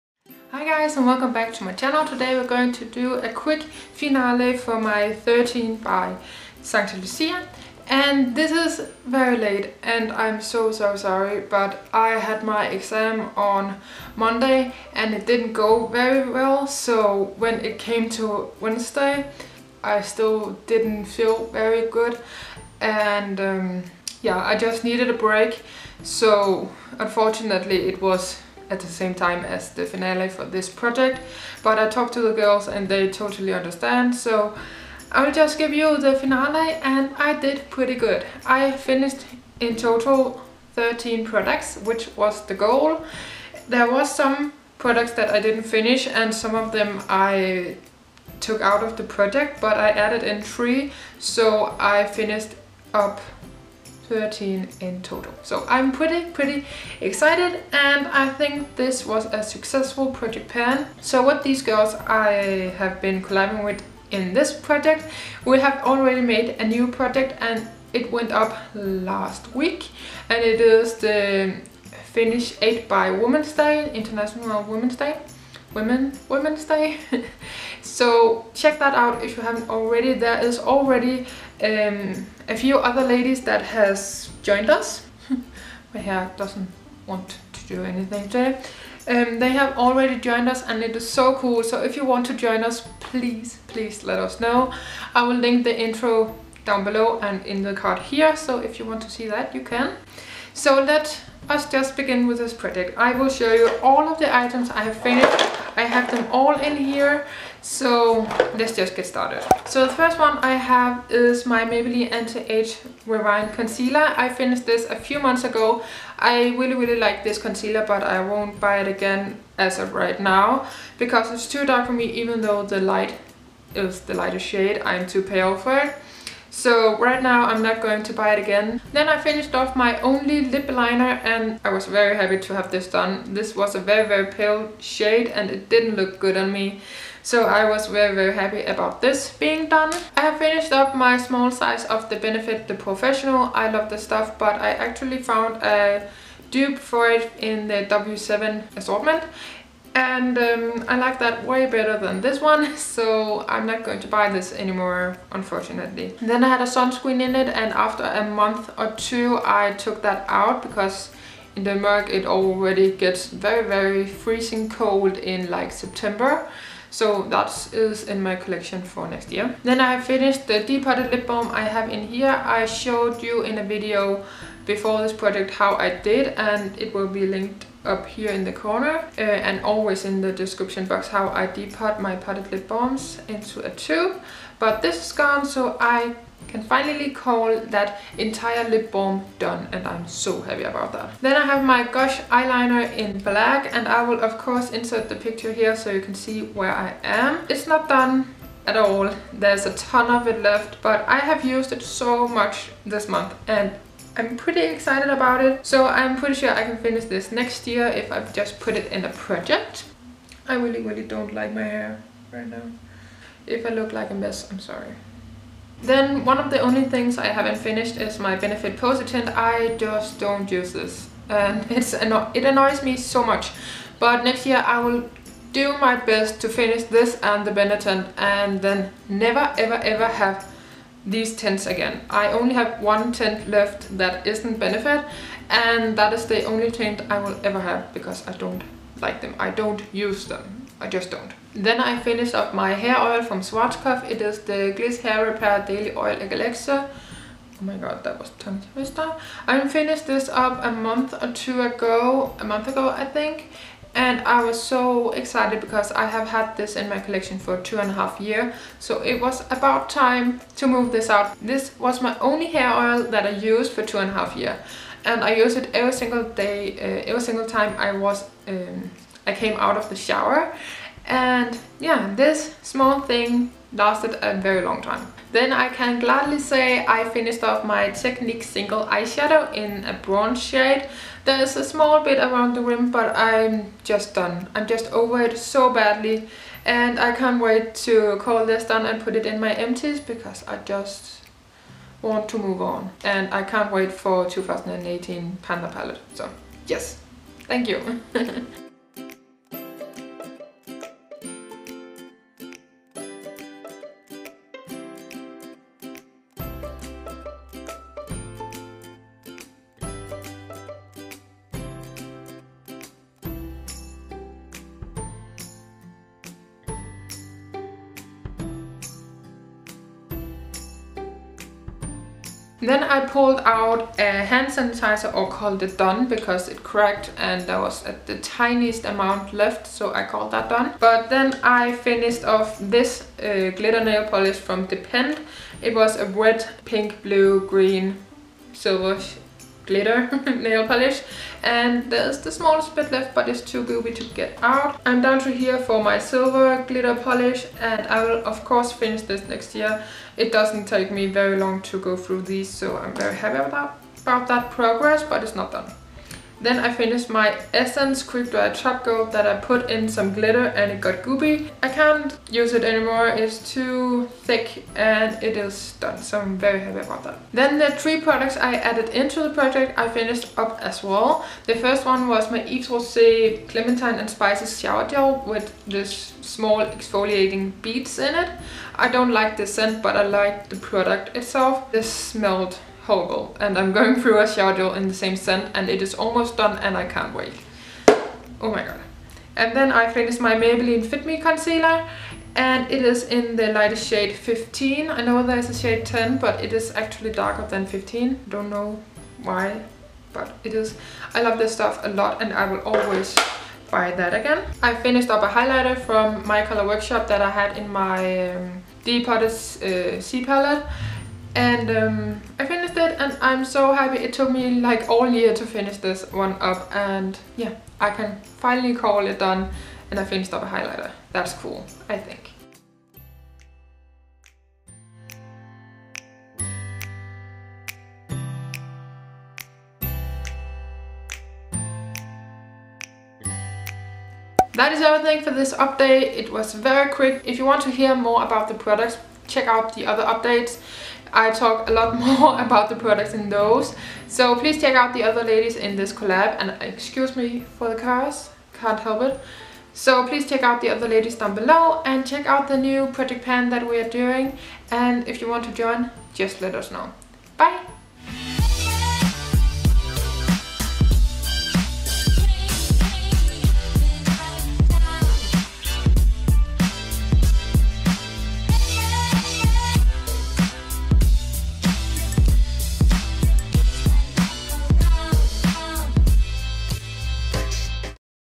FINISH 13 BY ST LUCIA | FINALE // COLLAB
PRODUCTS :

 1) Maybeline Instant anti age rewind consealer - ivory (DONE)
 2) Only Cosmetics lipliner (DONE)
 3) Benefit porefessional (DONE)
 4) Ombia LSF 30 - Sun lotion (DECLUTTERED)
 5) Lisbeth Dahl - Lipbalm (Depotted) (DONE)
 6) Gosh - Velvet touch eyeliner waterproof - 32 Black ink.
 7) Benefit Posietint
 8) Scwartzkopf Gliss hair repair daily oil elixir (DONE)
 9) Techniq depotted single - bronze (DONE)
 10) Dulcon Aloe Vera hand sanitizer (DONE)
 11) Depend Glitter polish - Red (DONE)
 12) Depend Glitter polish - Silver
 13) Essence quick dry top coat (DONE)

 14) Yves Rocher Exfoliating shower gel  (DONE)
 15) Maybeline fit me consealer - 15 (DONE)
 16) My color workshop highlighter (DONE)

The ladies :

Nanna : @NannasUnivers (Instagram)
Caroline : @beautyscandinavia (Instagram)

Pan that palette & Project pan 2016

Pan that palette & Project pans 2015 (danish)

M U S I C:
 - YOUTUBE AUDIOLIBrARY